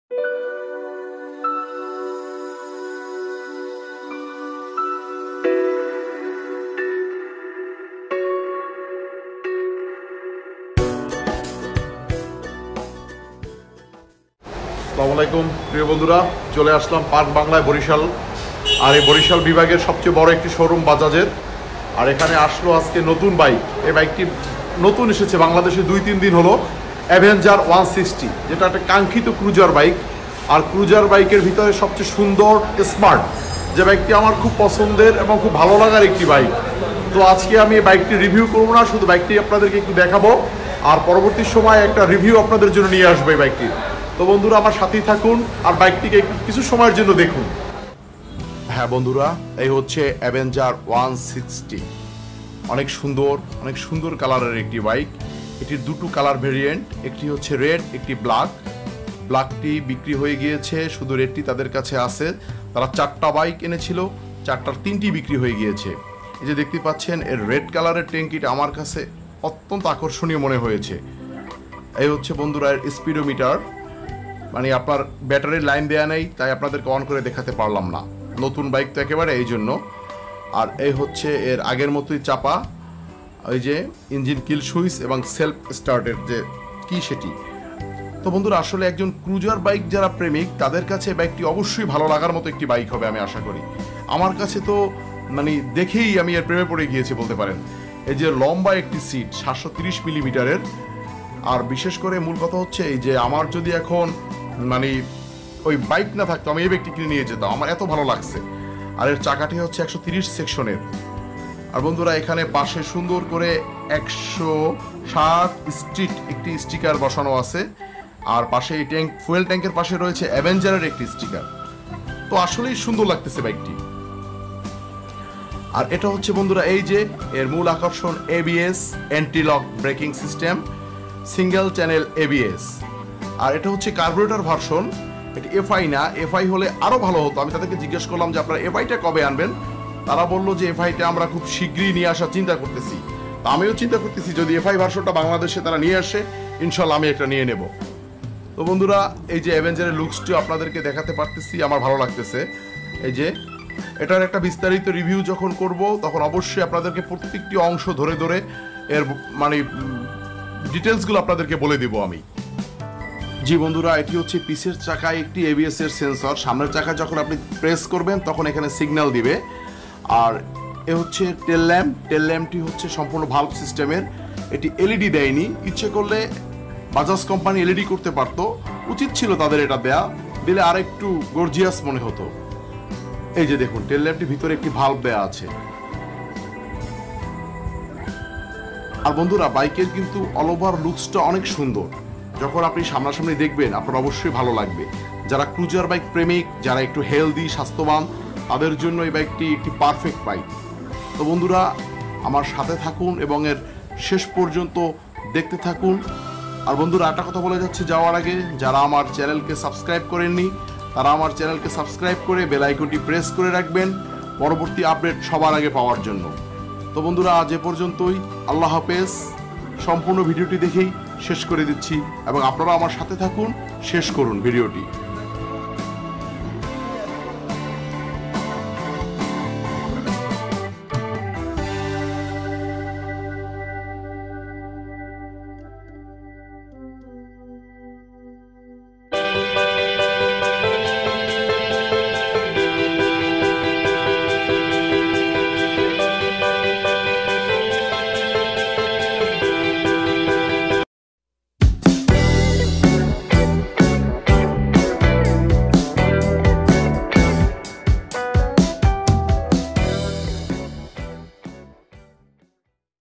Bajaj Avenger 160 Street first impression review 2022।।Bajaj Avenger 160 Street।price specification।
Bajaj Avenger 160 first looks।Bajaj Avenger 160 Street।।

Avenger 160 নতুন এসেছে আমাদের বাংলাদেশে, অনেক সুন্দর একটি বাইক, আমি ভিডিও টি করেছি বরিশাল পার্ক বাংলা তে তাদের ফোন নং নিচে দিলাম। শাহিন ভাই তার সাথে যোগাযোগ করলে বিস্তারিত জানতে পারবেন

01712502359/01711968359